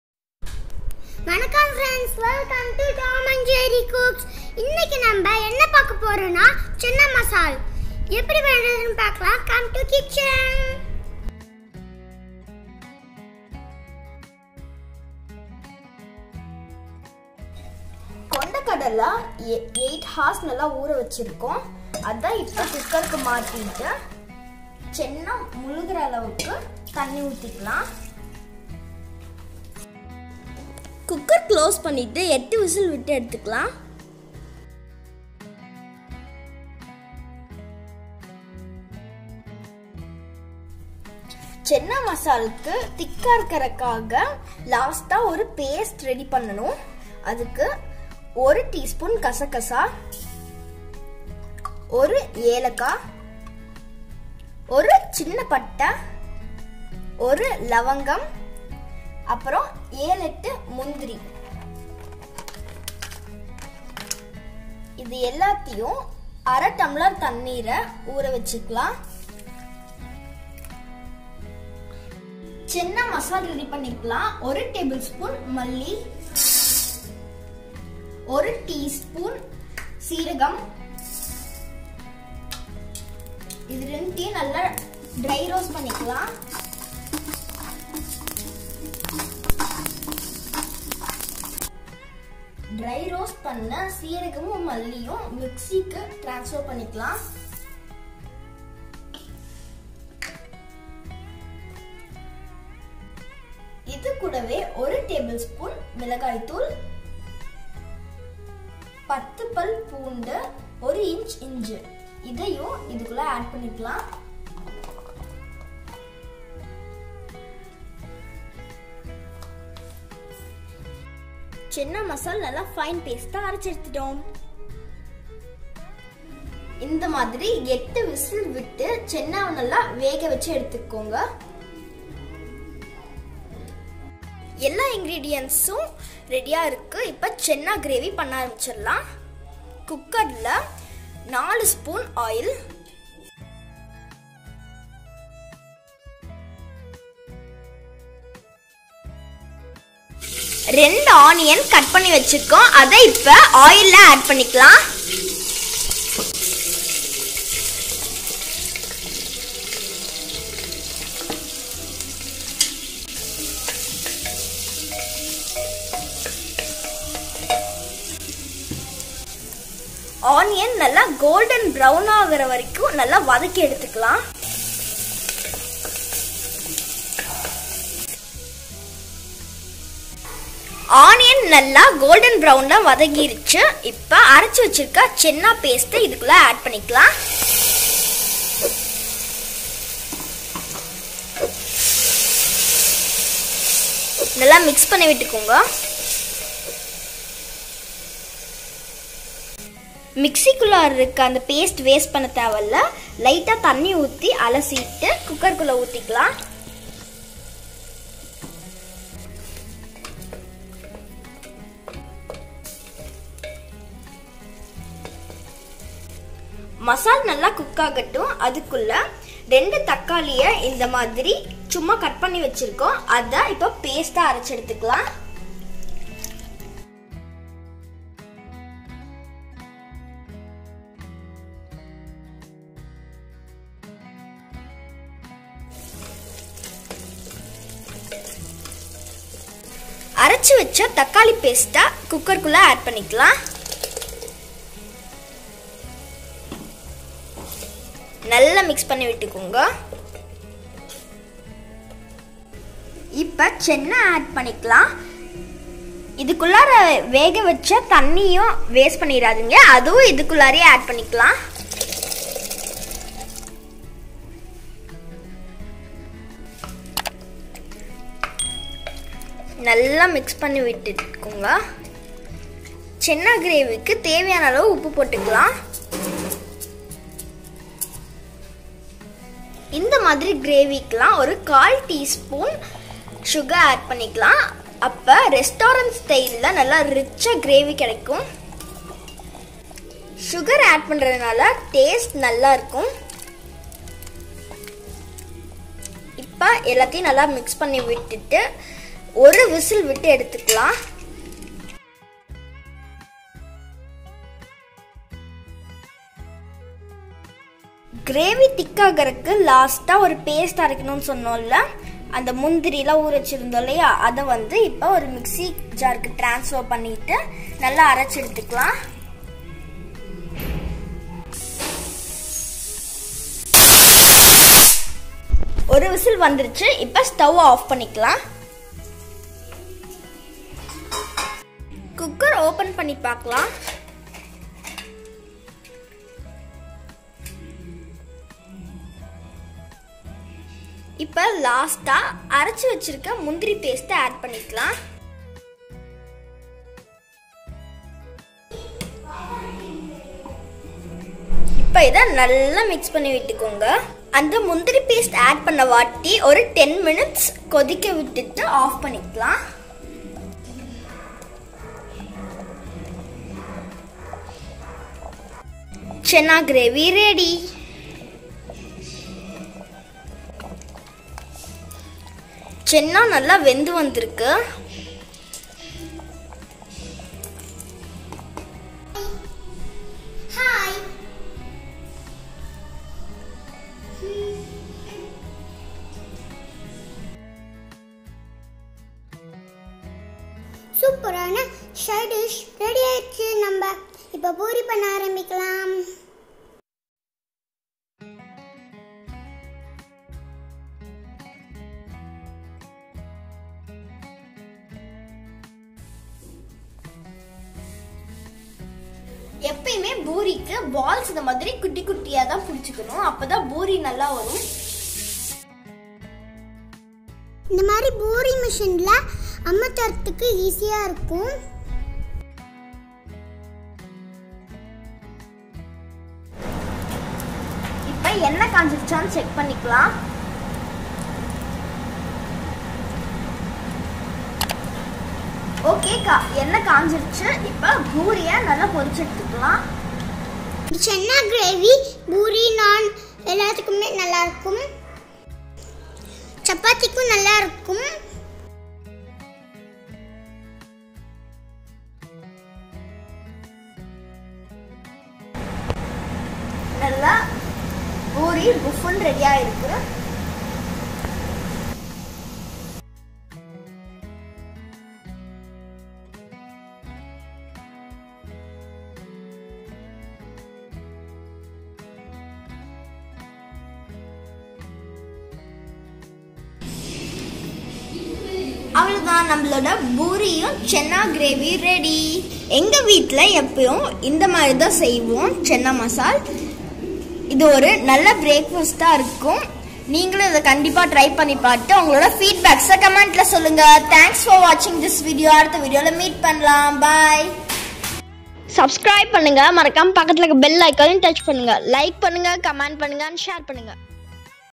Channa Masala Recipe In Tamil ||சென்னா மசாலா ||Chapati Side Dish Masala
Here comes the Tasty Channa masala recipe from Tom and Jerry cooks. This is an all time favourite side dish for Poori, Chapati & Roti. Try this easy recipe and comment your feedback...